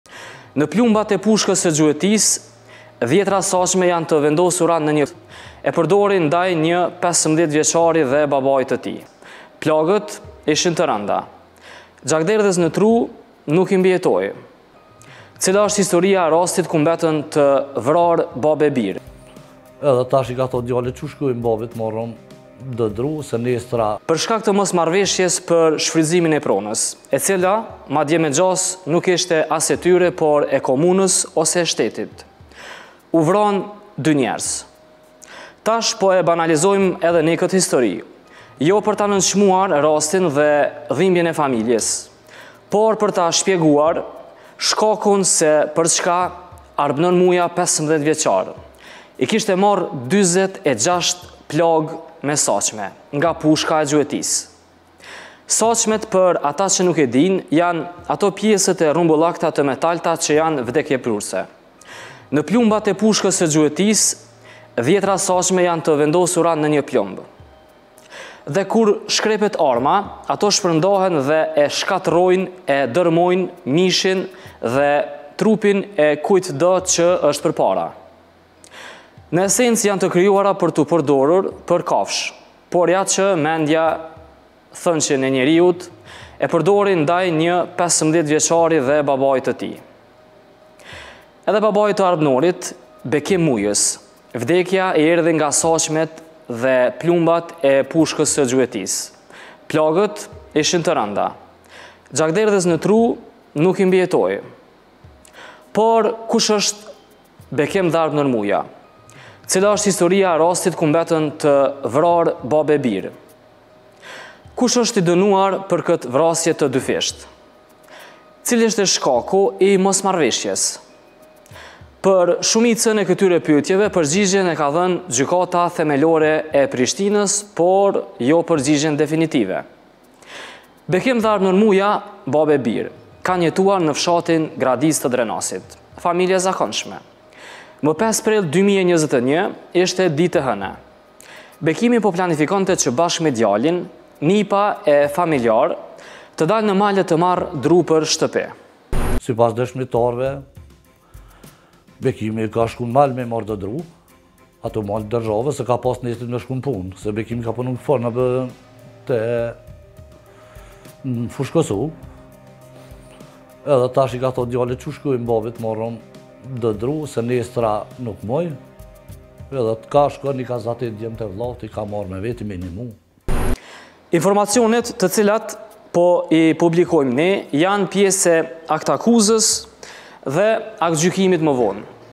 Kush është Bekim dhe Arbnor Muja që u vranë në një mal të Gradicës
“15 vjeçari kishte marrë 46 plagë”,”I tha djalit cu shkojna i marrim do dru”/ Kush është Bekim dhe Arbnor Muja që u vranë në një mal të Gradicës ➖➖➖
Për më shumë na ndiqni në: